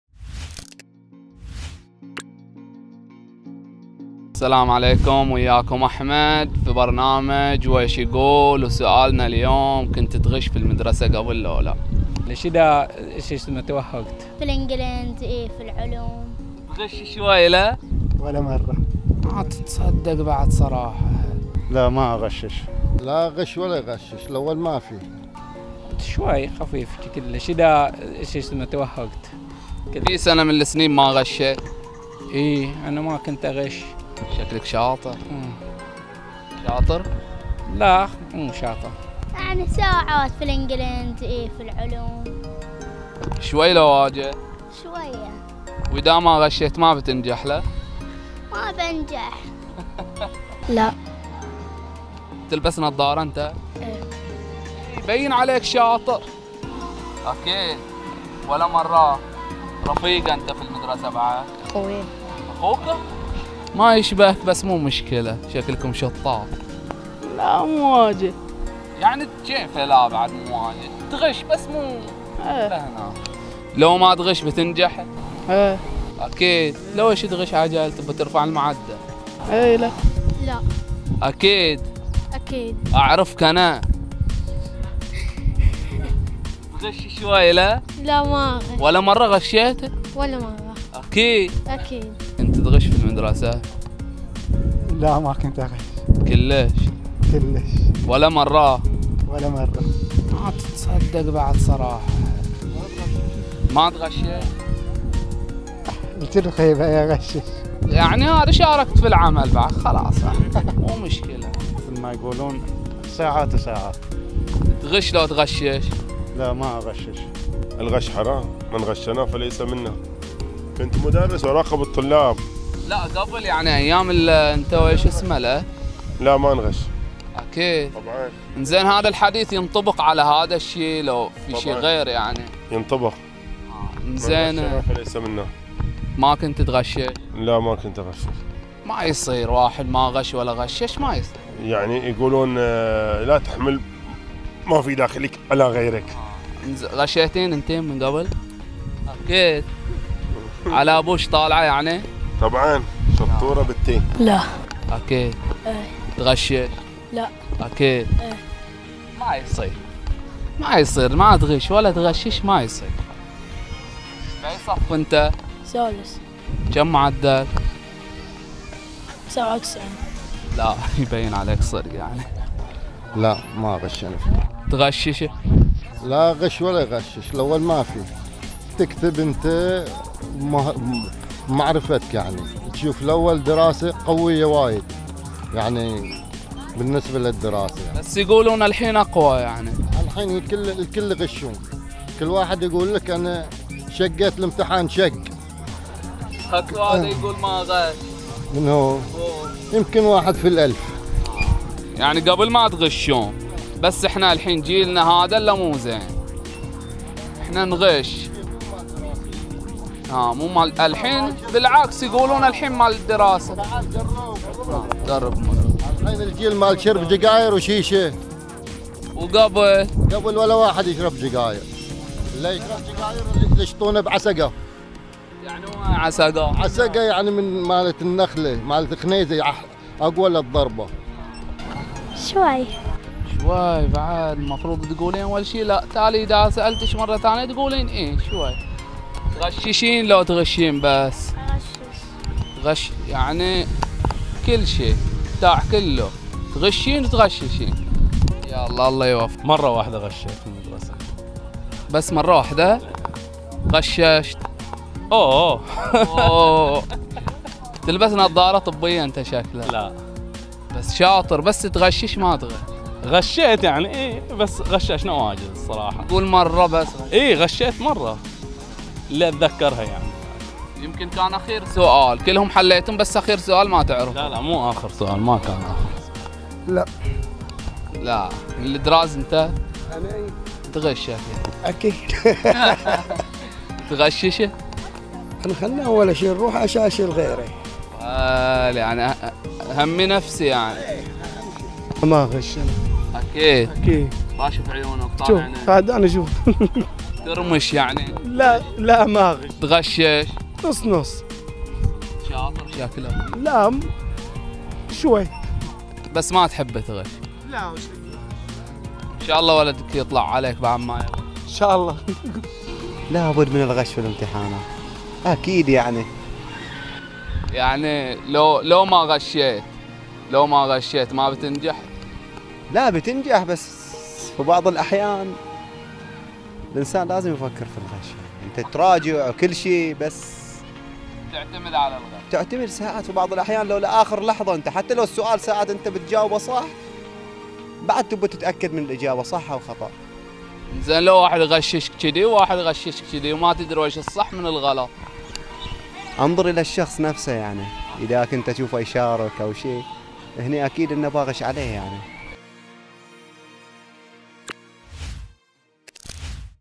!الغش في الامتحانات من صوبك ؟ | ويش يقول ؟
احمد يرحب بكم في حلقة جديدة من برنامج ويش يقول، وسؤالنا اليوم : كنت تغش في المدرسة قبل لو لا ؟

نرجو الدعم للمزيد من مقاطع الفيديو